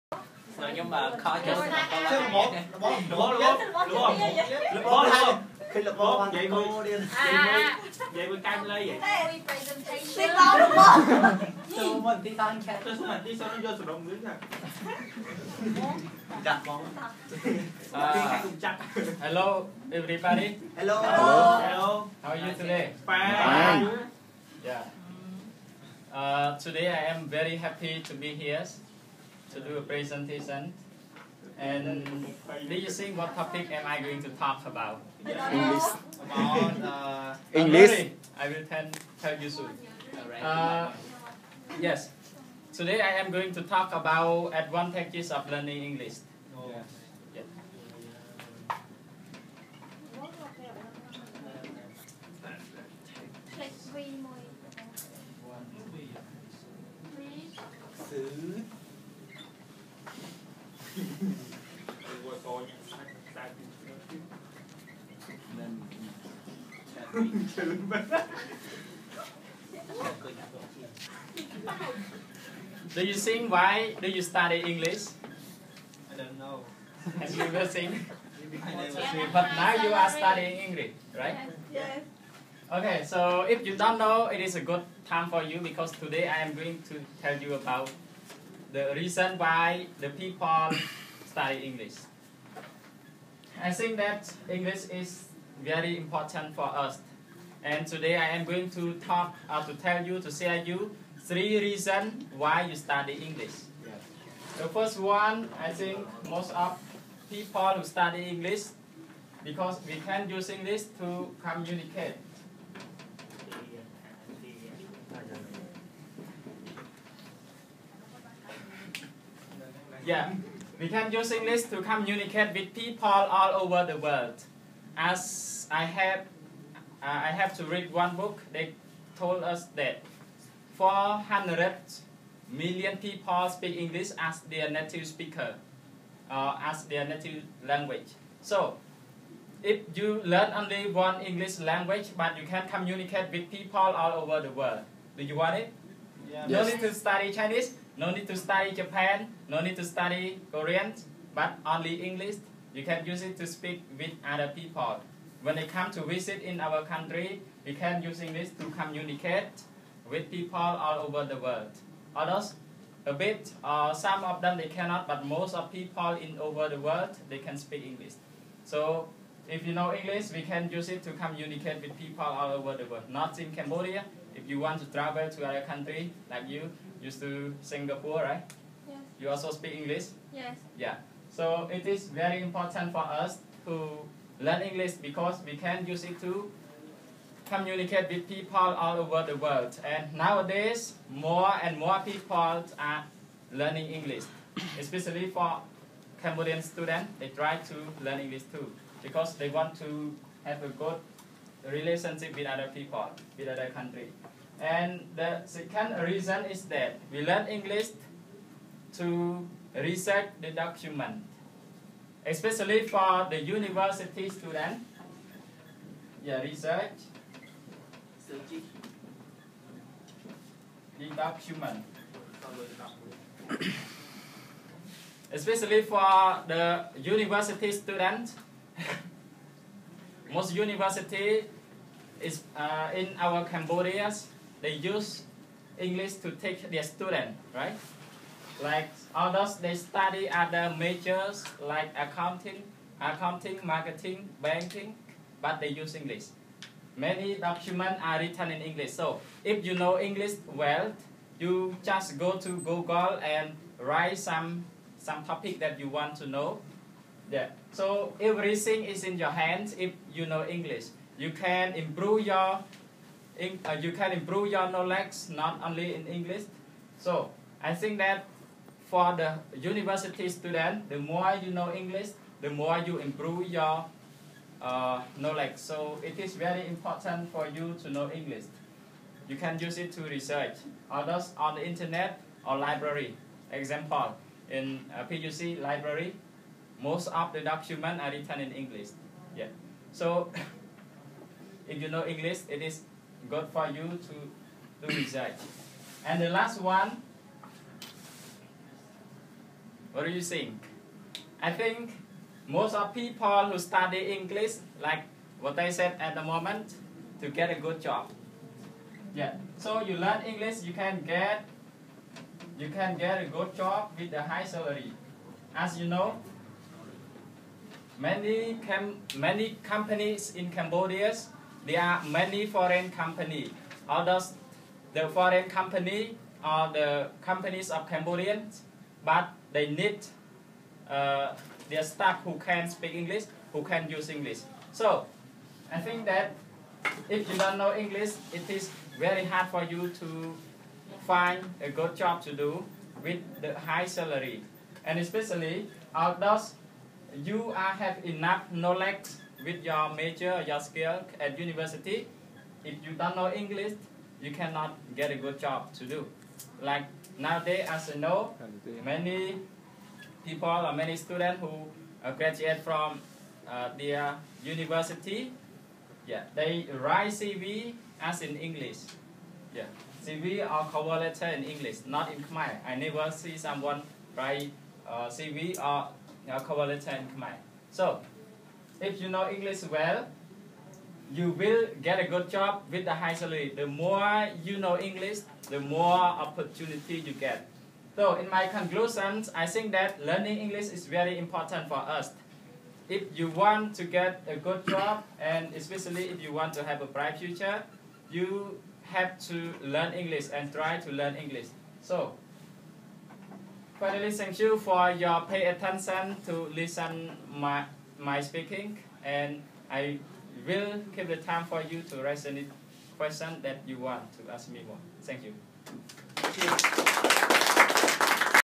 Advantages of Learning English | PROEUNG THOY
Advantages of Learning English, spoken by venerable PROEUNG THOY, Pannasastra University of Cambodia's student. November, 2015.